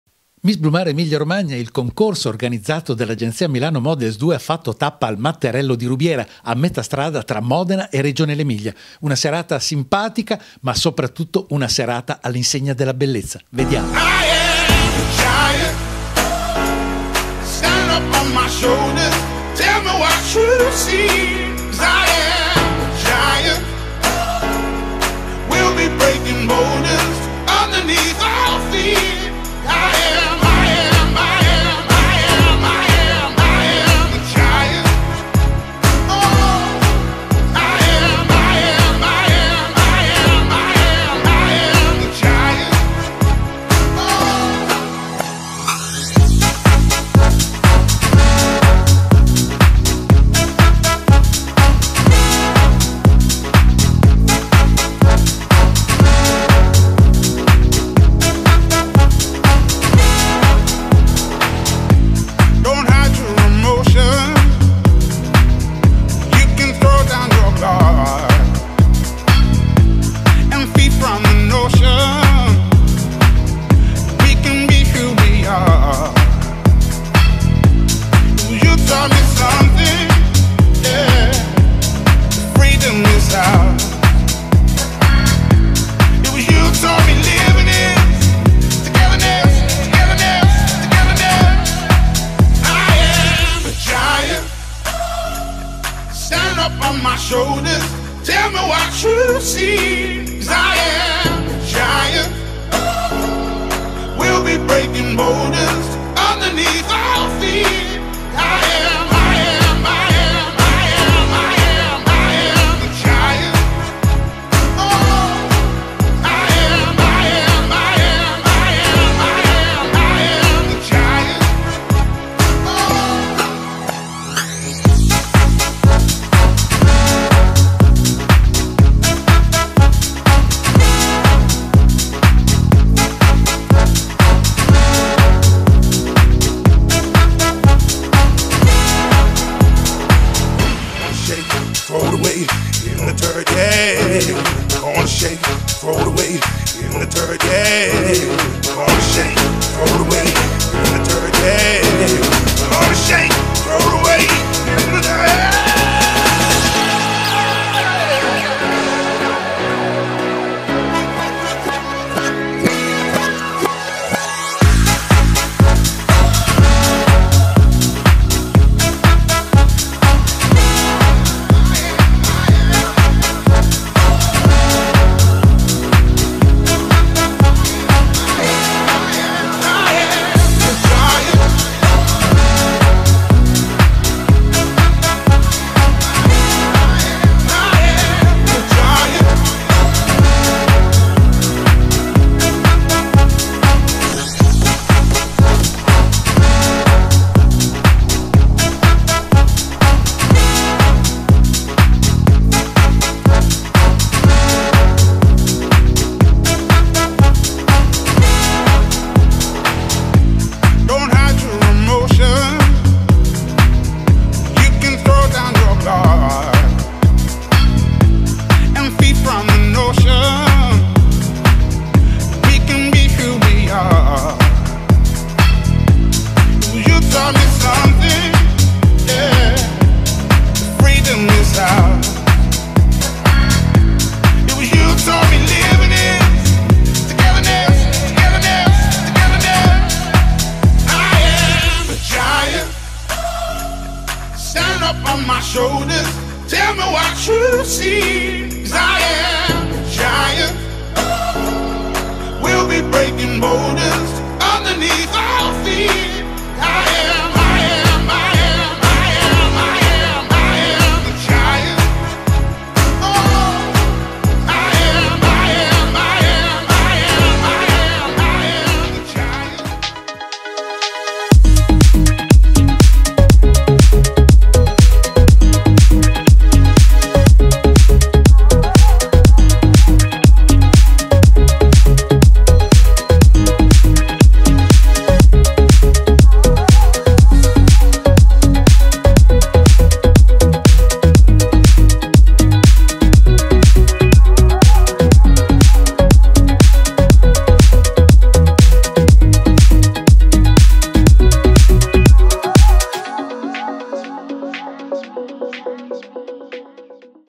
Miss Blumare 2019 Emilia R. - Selezione "Matte" Disco Show & Dinner - Rubiera (Re) - 08.02.2019
Nel video, realizzato da Claudio Maria Goldoni, la selezione per l'Emilia-Romagna  del concorso "Miss Blumare 2019" (partner regionale agenzia Milano Models 2 di Modena diretta da Paolo Zaccaria) che si è tenuta venerdì 8 febbraio 2019 all'interno del "Matte" Disco & Dinner di Rubiera (ex Matterello) diretto da Sauro Malavasi. Hanno partecipato alla serata, presentata dal giornalista Massimo Manaresi, le seguenti concorrenti (in ordine di apparizione): Agata Perla Lucchini, Alessia Canovi, Elena Batireanu, Ludovica Montorsi, Deborah Morciano e Asia Ferretti. Al primo posto si è classificata la studentessa Ludovica Montorsi